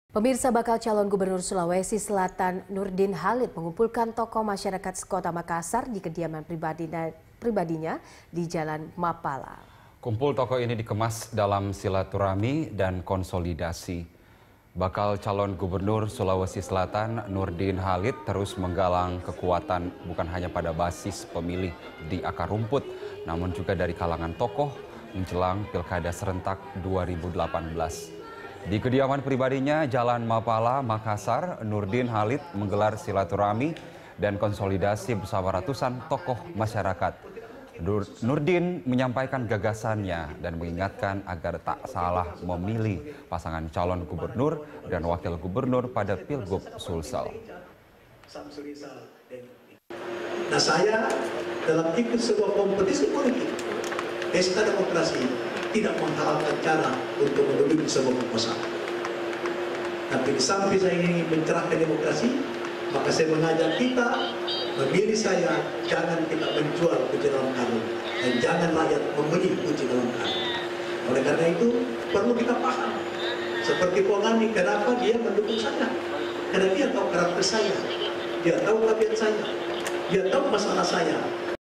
Nurdin Halid Kumpulkan Tokoh Masyarakat di Makassar
Bakal calon gubernur Sulawesi Selatan, Nurdin Halid mengumpulkan tokoh masyarakat se-Kota Makassar di kediaman pribadinya, Jalan Mapala. Kumpul tokoh ini dikemas dalam silaturahmi dan konsolidasi.

Facebook.com/BeritaSatuTV
Youtube.com/BeritaSatu